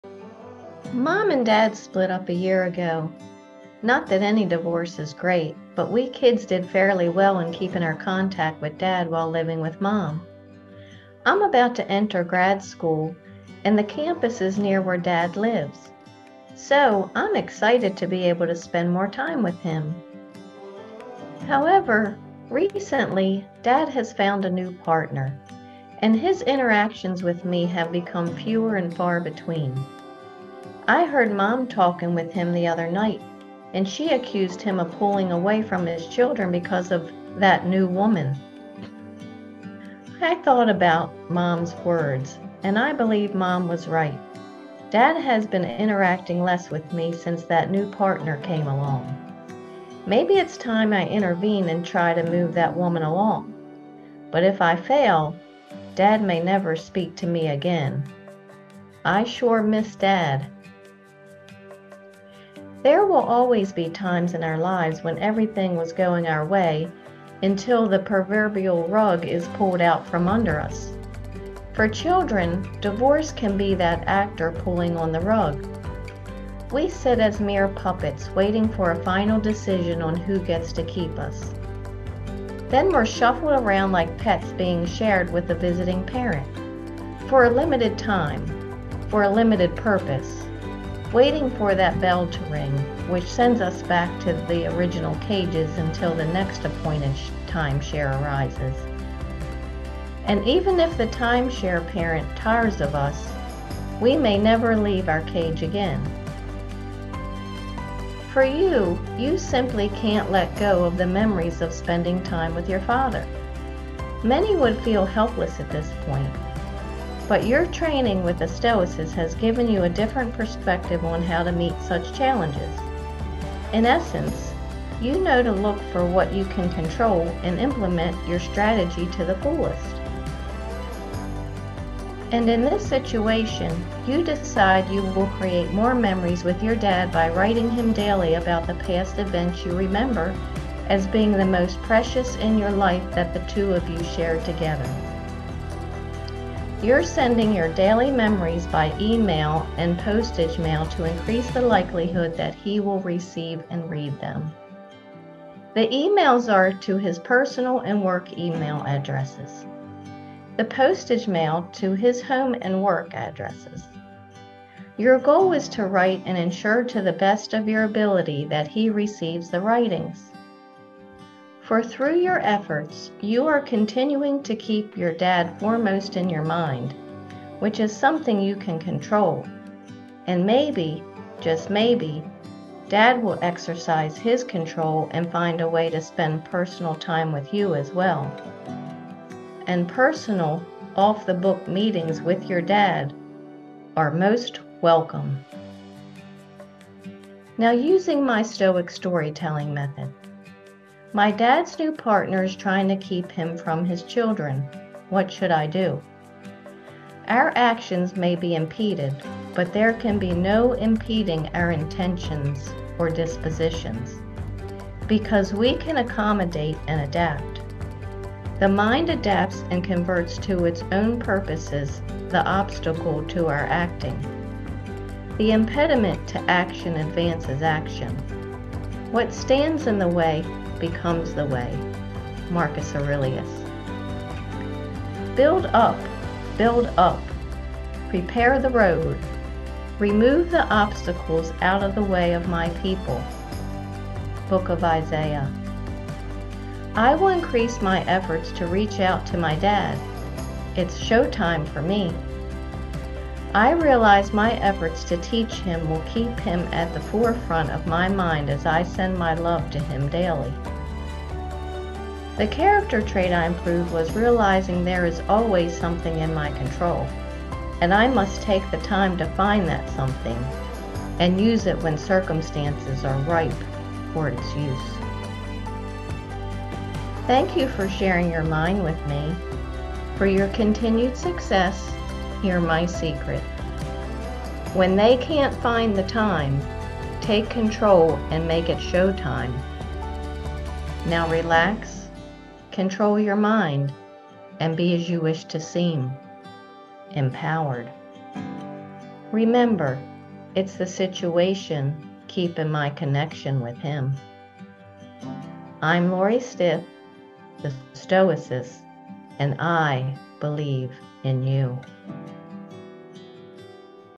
Dads partner blocks children
Dad remarried after the divorce, and his new partner doesn't want him to see us kids. But I figured out a way. Listen to the podcast for a success story.